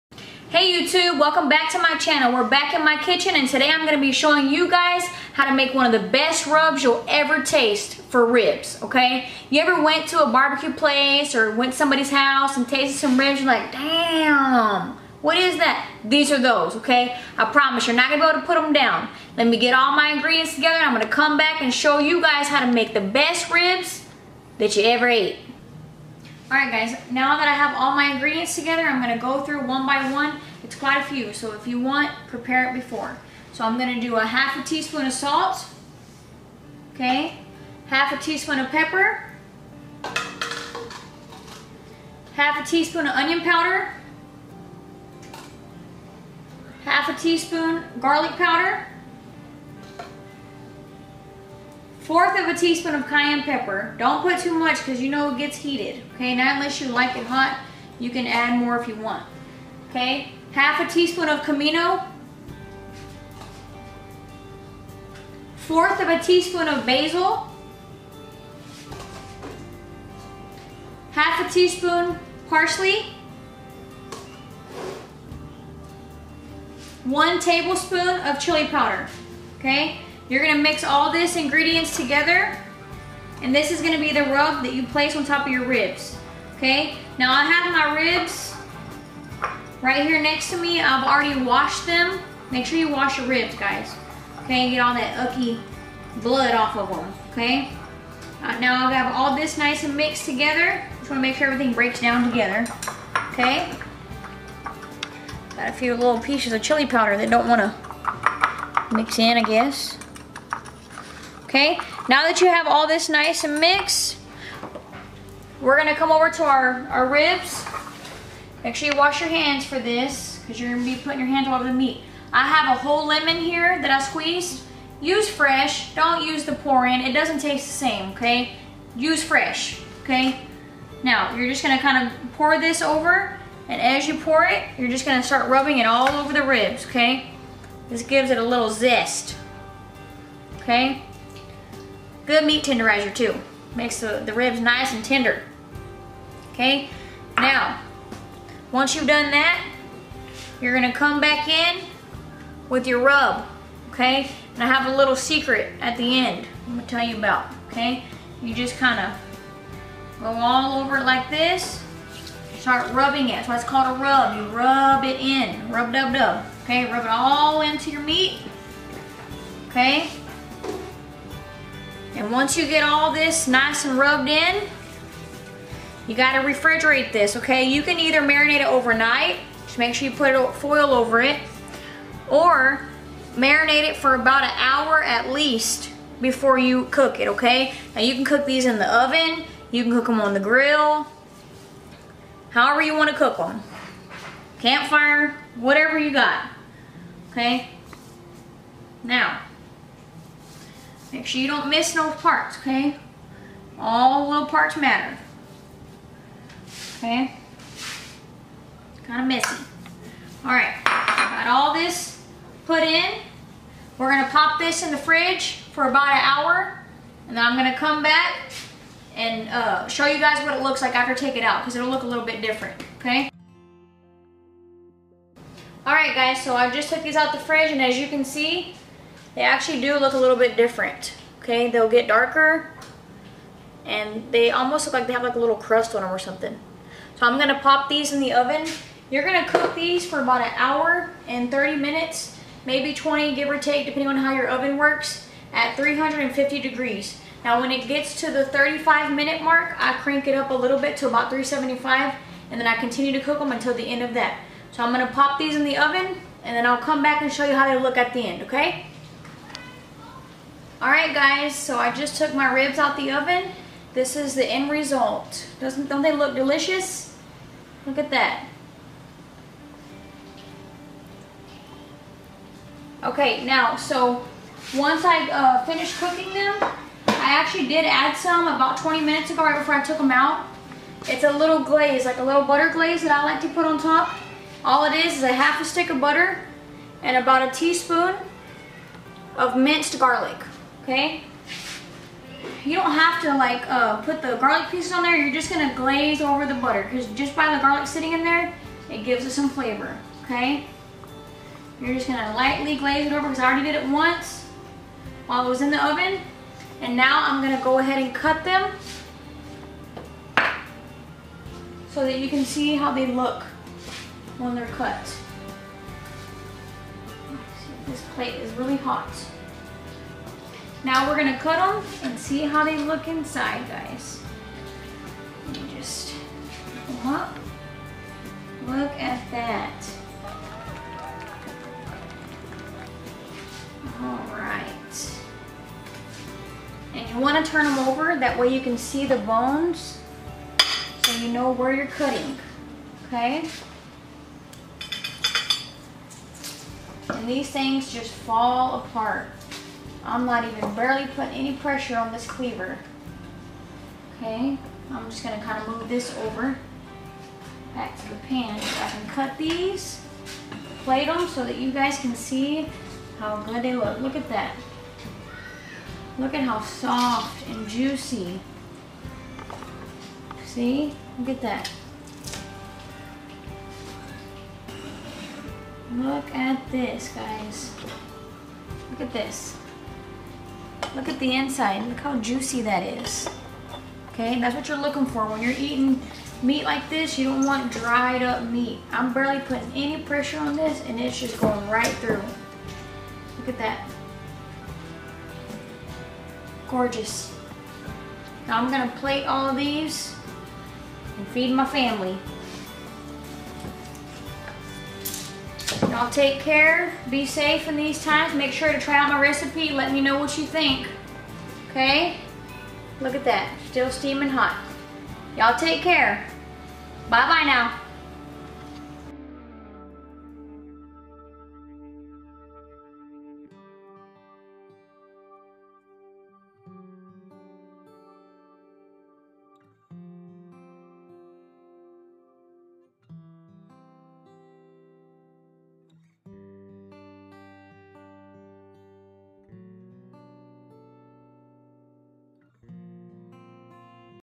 How to make BBQ RIBS in the oven|BEST RECIPE😋 FALL off the BONE!!!
Ribs best recipe..Best dang rib rub recipe you ever ate! These ribs will leave your tastebuds happy as they are packed full of savory flavors.. you can top these with your favorite BBQ sauce or you can eat just like this either way you won’t be disappointed.. thanks for watching!!

Ingredients

1/2 tsp salt
1/2 tsp Black pepper
1/2 tsp  Onion powder
1/2 tsp  Garlic powder
1/4 tsp  Cayenne pepper ( add more if desired)
1/2 tsp Cumino
1/4 tsp Basil
1/2 tsp Parsley
1 tbsp Chili powder

Directions
Preheat oven to 350
Combine all ingredients listed above
rub into ribs
chill for 1 hour before cooking or let marinate over night
bake at 350 for 30 minutes then turn up to 375 for remainder of time (1hr 30min total)
watch video till end for secret ingredient

* Yoiu can add BBQ sauce if desired!!

*slice bones up and plate*

. Then just serve  and enjoy!